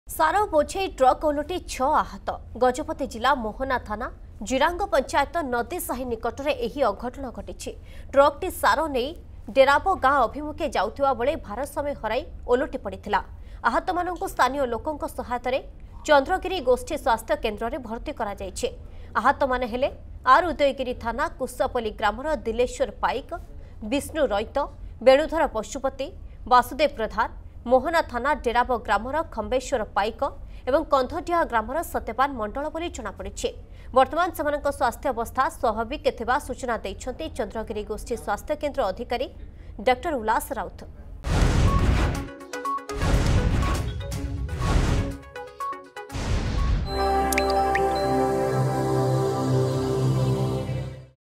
Gajapati: ସାର ବୋଝେଇ ଟ୍ରକ୍ ଓଲଟି ୬ ଆହତ | Fertilizer Carrying Truck Overturns, 6 Injured
ସାର ବୋଝେଇ ଟ୍ରକ୍ ଓଲଟି ୬ ଆହତ। ଗଜପତି ଜିଲ୍ଲା ମୋହନା ଥାନା ଜିରାଙ୍ଗ ପଞ୍ଚାୟତ ନଦୀସାହି ନିକଟରେ ଏହି ଦୁର୍ଘଟଣା ଘଟିଛି।  ଟ୍ରକଟି ସାର ନେଇ ଡେରାବ ଗାଁ ଅଭିମୁଖେ ଯାଉଥିବା ବେଳେ ଭାରସାମ୍ୟ ହରାଇ ଓଲଟି ପଡିଥିଲା।